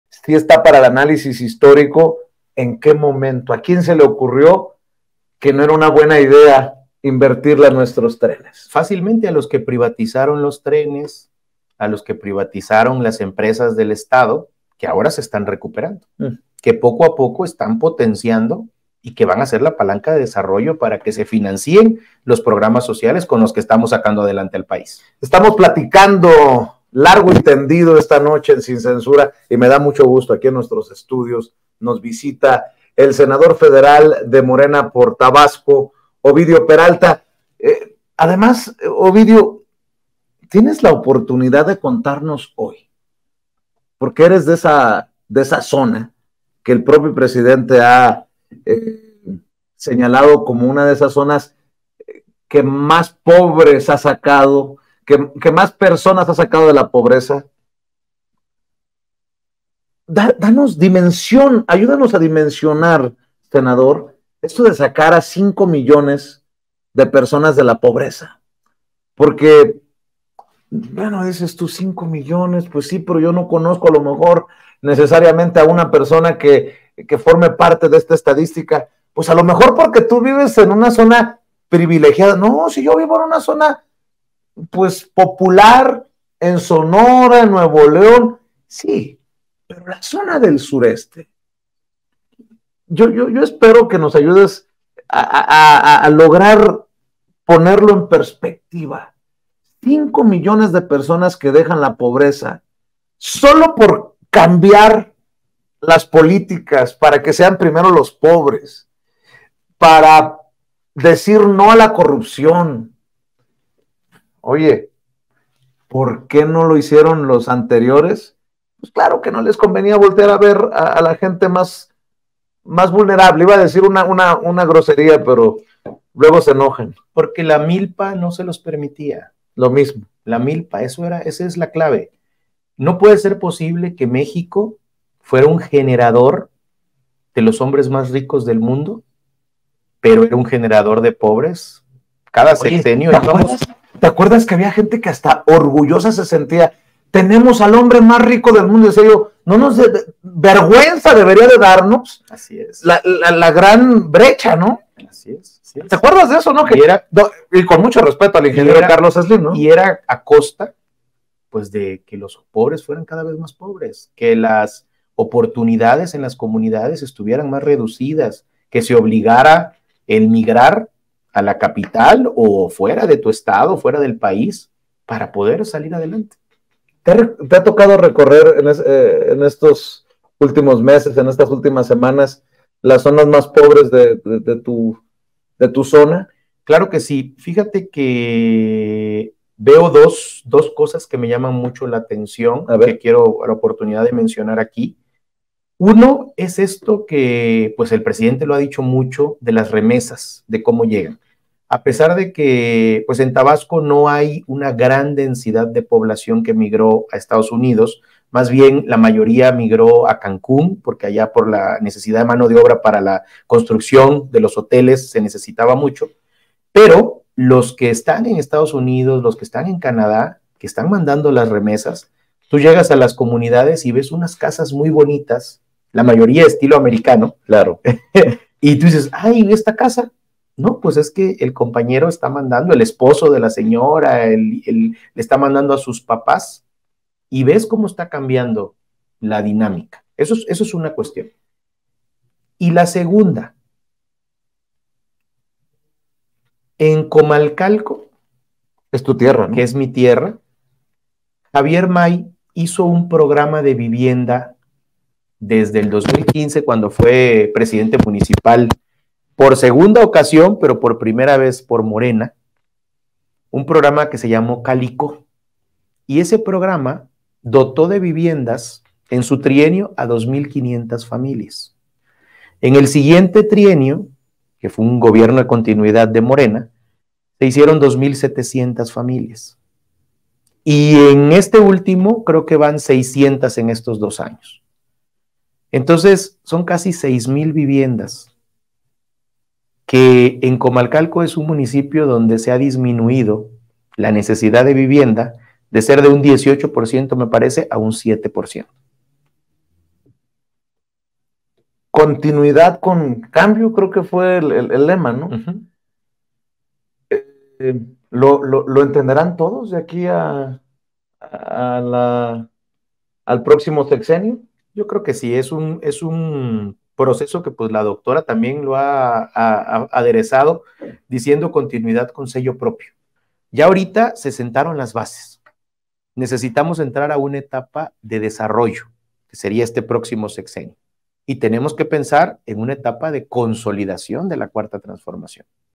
LA “MILPA” IMPIDIÓ QUE LOS GOBIERNOS ANTERIORES ¡PUDIERAN SACAR A LOS MEXICANOS DE LA POBREZA!
Ovidio Peralta reflexiona sobre el logro que representa haber sacado de la pobreza a cinco millones de mexicanos durante el sexenio del presidente Andrés Manuel López Obrador y señala que lo único que impedía que este fenómeno ocurriera en gobiernos anteriores ¡era la milpa!

Ovidio Peralta Suárez: Senador de Morena